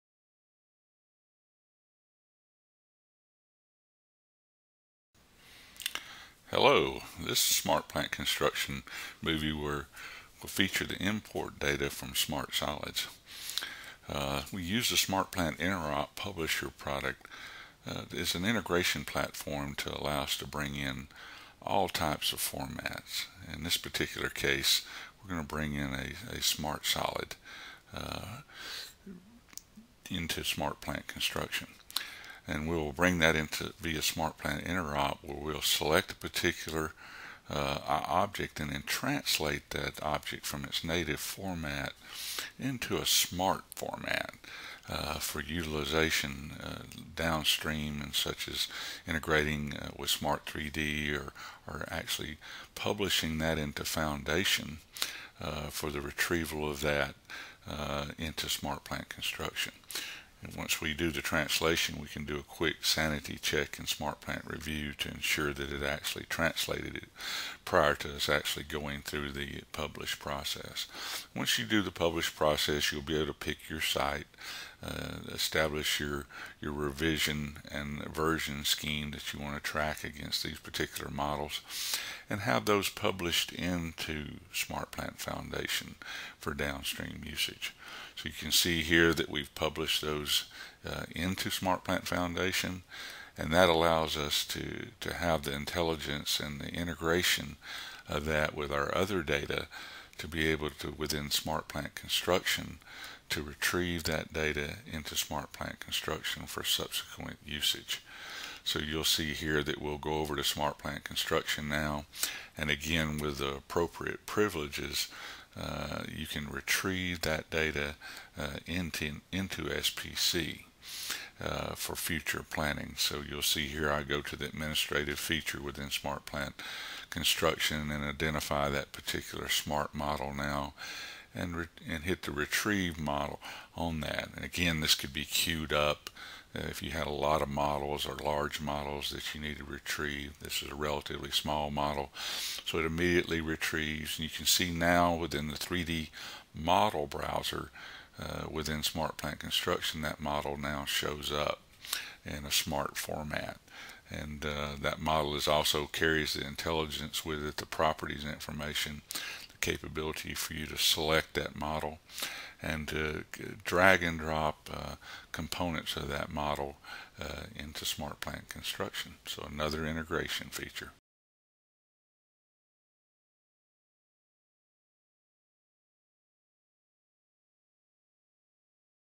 Intergraph SmartPlant Construction Importing Data from SolidWorks
This video features the import data from Smart Solids using  SmartPlant Interop Publisher and how SmartPlant Construction can retrieve this information.